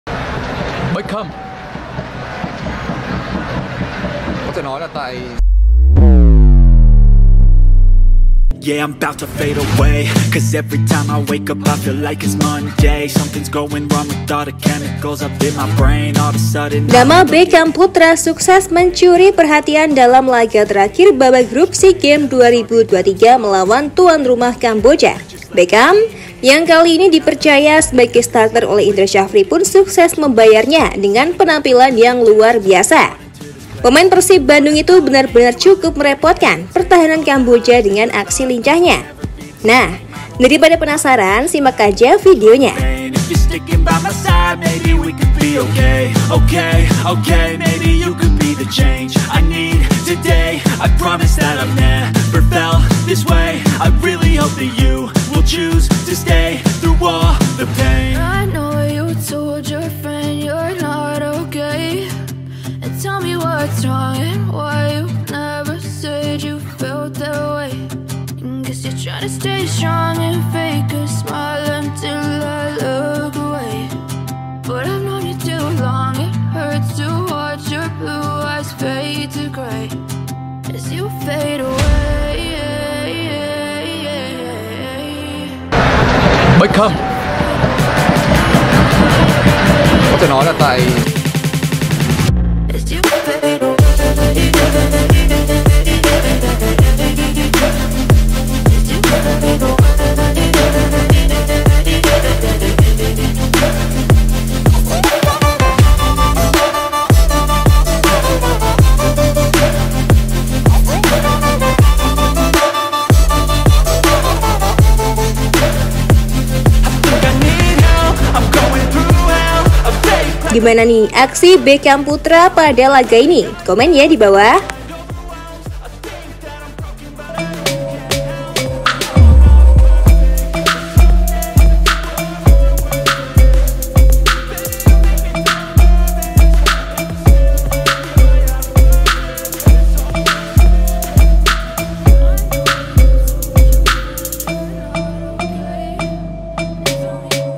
INDONESIA VS KAMBOJA ‼️Full Pembuktian Beckham Putra Hancurkan Kamboja di Final Group Sea Games 2023
Nama Beckam Putra sukses mencuri perhatian dalam laga terakhir babak grup Sea Games 2023 melawan tuan rumah Kamboja. Beckam yang kali ini di percaya sebagai starter oleh Indra Sjafri sukses membayarnya dengan penampilan yang luar biasa. Pemain Persib Bandung itu benar-benar cukup merepotkan pertahanan Kamboja dengan aksi lincahnya.

Saksikan aksi dari Beckham Putra selengkapnya di dalam Video ini!